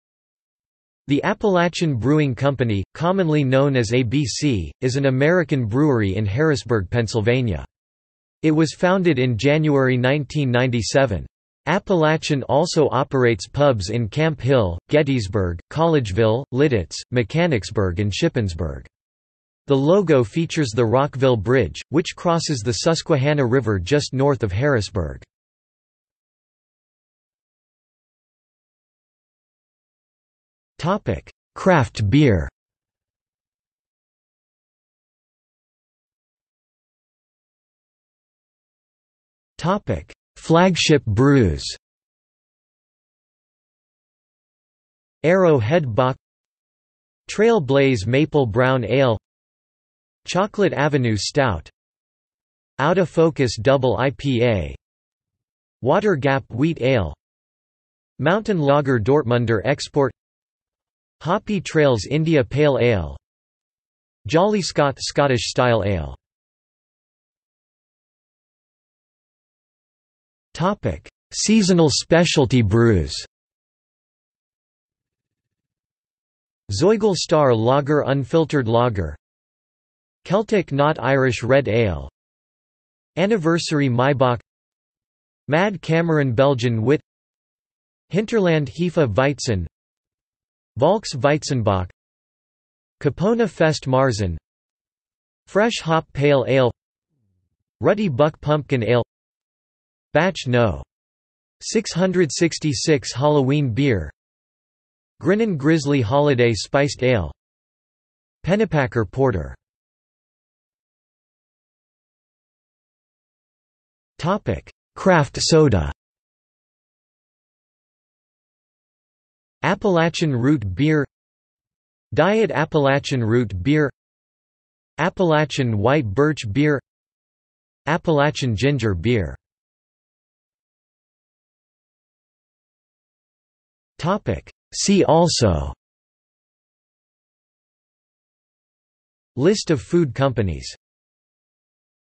Appalachian Brewing Company | Wikipedia audio article
00:00:27 1 Craft beer
00:00:36 1.1 Flagship brews
00:01:07 1.2 Seasonal specialty brews
00:01:50 2 Craft soda
00:02:10 3 See also

- Socrates

SUMMARY
The Appalachian Brewing Company, commonly known as ABC, is an American brewery in Harrisburg, Pennsylvania. It was founded in January 1997. Appalachian also operates pubs in Camp Hill, Gettysburg, Collegeville, Lititz, Mechanicsburg and Shippensburg. The logo features the Rockville Bridge, which crosses the Susquehanna river just north of Harrisburg.